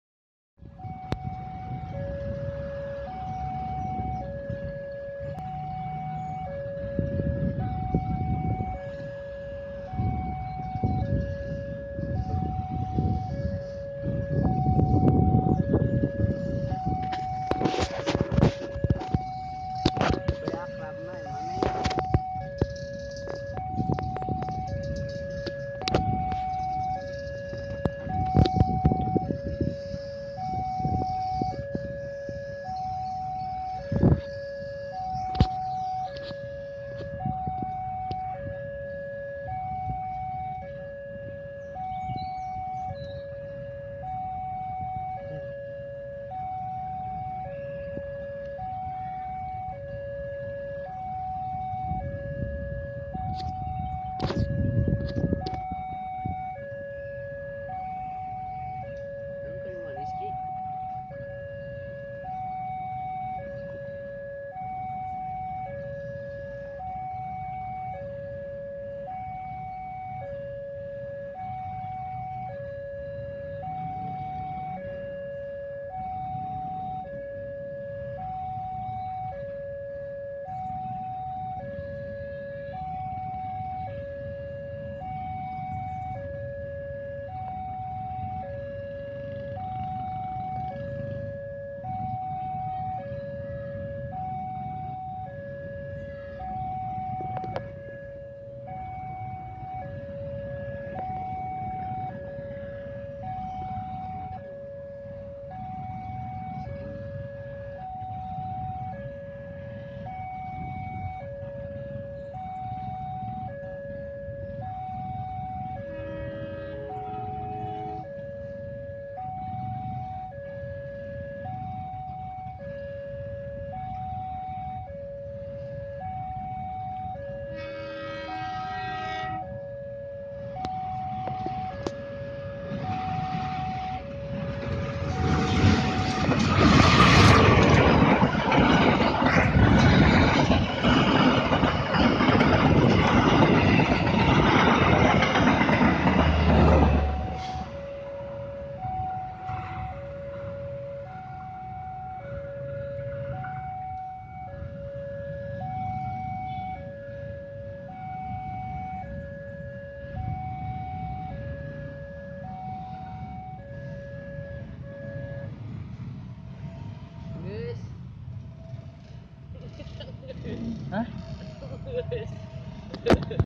Perlintasan kereta api Gede bage
Spesial Camera, -Kereta api yang melintas-Ka lokal Bandung Raya Padalarang Cicalengka